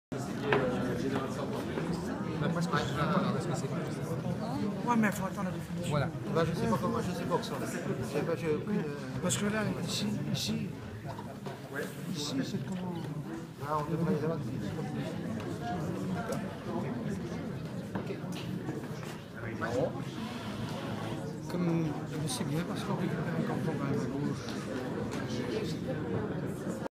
Elections départementales à Fougères
Elections départementales : résultats à Fougères avec les candidats de Fougères 1 et Fougères 2